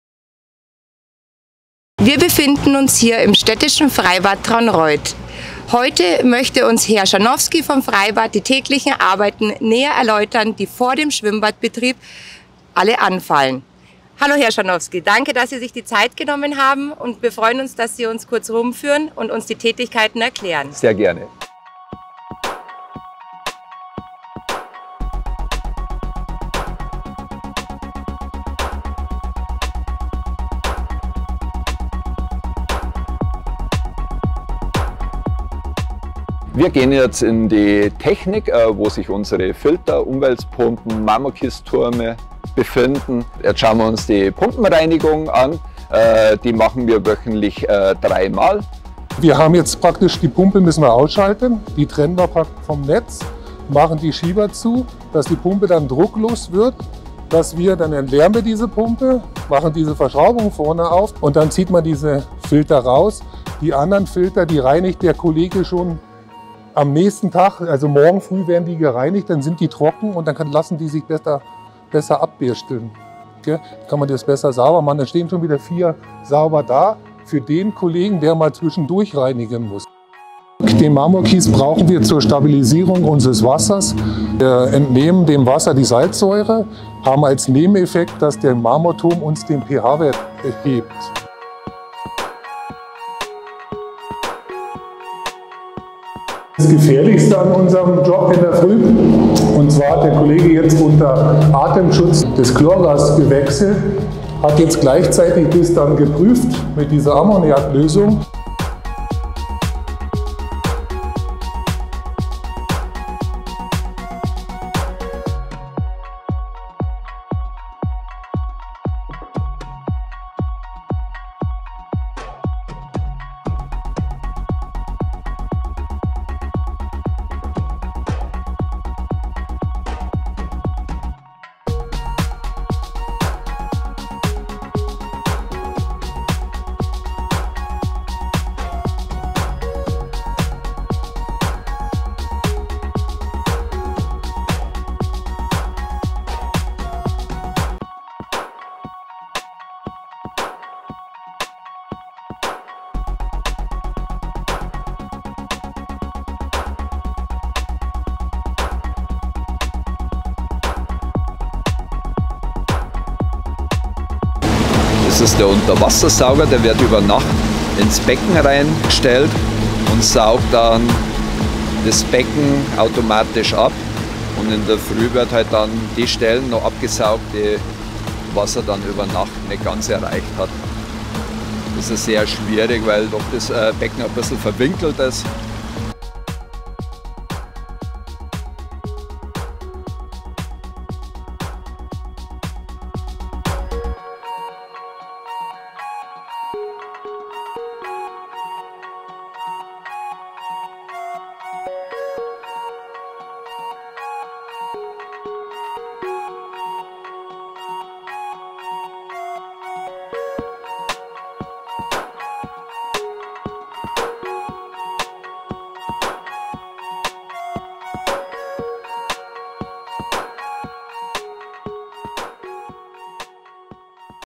Vorbereitung Freibad - Hinter den Kulissen
Ein Blick hinter die Kulissen des Franz-Haberlander-Freibades Traunreut:
Iris Alesch, Mitarbeiterin der Stadtverwaltung Traunreut, begibt sich mit Wolfgang Chrzanowski und Ralf Nerger , beide Mitarbeiter der Stadtwerke Traunreut, auf einen Rundgang in die Arbeits- und Technikbereiche des Schwimmbades. Hier zeigen uns die beiden Mitarbeiter an Beispielen welche Arbeiten nötig sind, um den Schwimmbadbetrieb vorschriftsmäßig durchführen zu können.
Übrigens: Fachkraft für Bäderbetriebe ist auch ein Ausbildungsberuf bei den Traunreuter Stadtwerken!